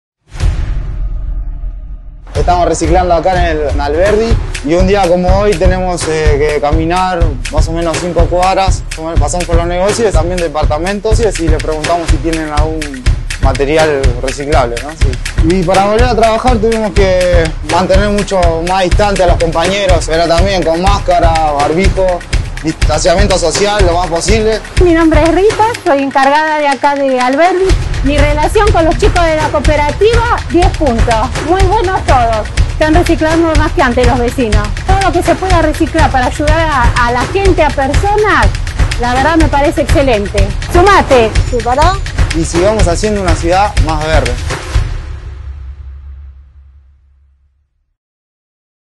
El reciclaje es uno de nuestros mayores compromisos con la Ciudad y el planeta
Recuperar materiales que desechamos para darles una nueva vida a través del reciclaje, es uno de nuestros más grandes compromisos con la Ciudad y con nuestro planeta.
Los recuperadores urbanos cumplen un rol clave en el proceso de recolección de los materiales reciclables. Gracias a su labor, logramos ampliar las toneladas de reciclables recolectados y nos vinculamos, a través de la promoción y concientización, con los vecinos y vecinas de la Ciudad.
En este video te mostramos esta parte fundamental del plan de higiene urbana.
¿Vos ya separás en casa?